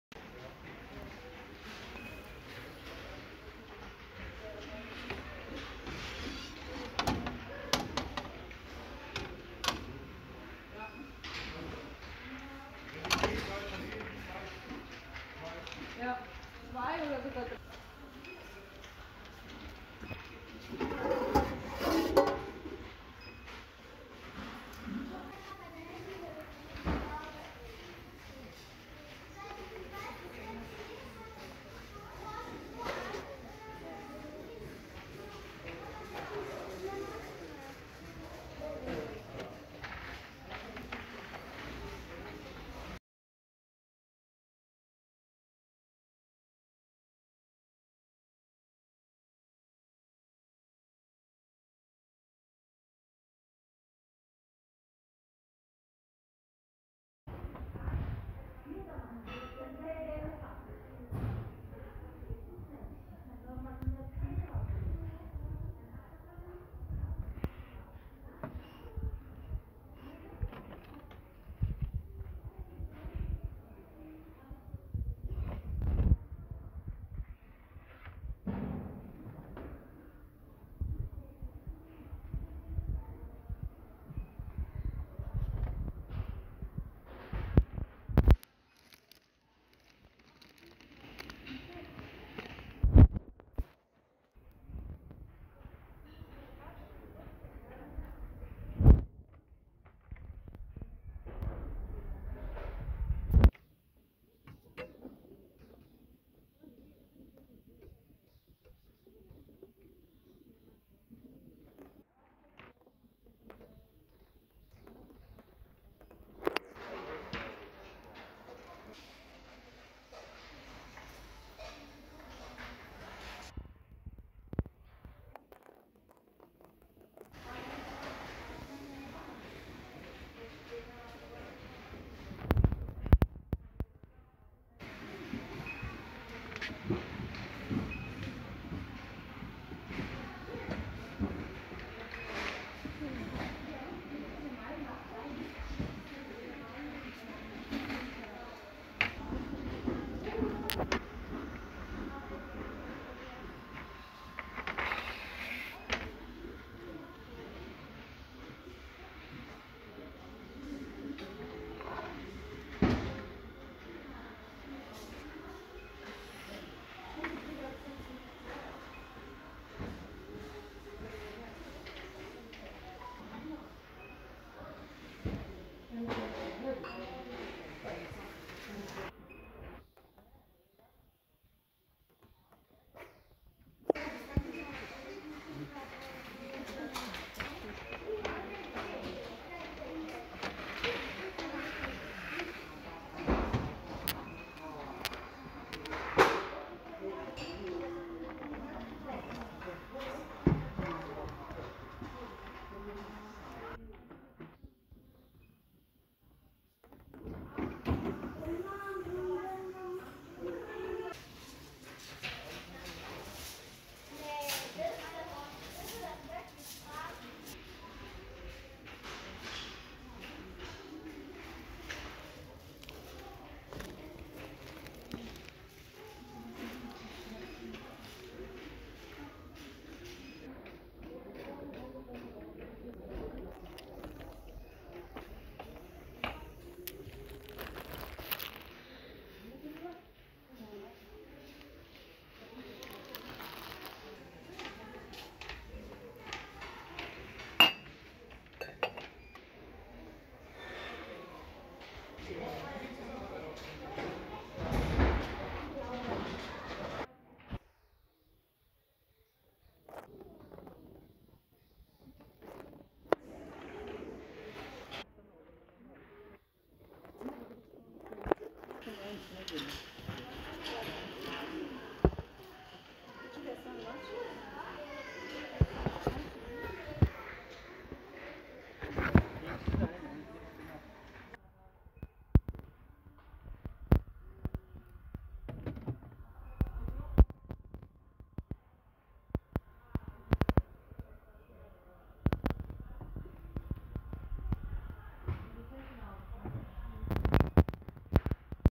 جولة خفيفة لهذا اليوم 27.05 في اكسيونAction لمعرفة الجديد و التخفيضات الموجودة حاليا😍
unbezahlt wernung ! .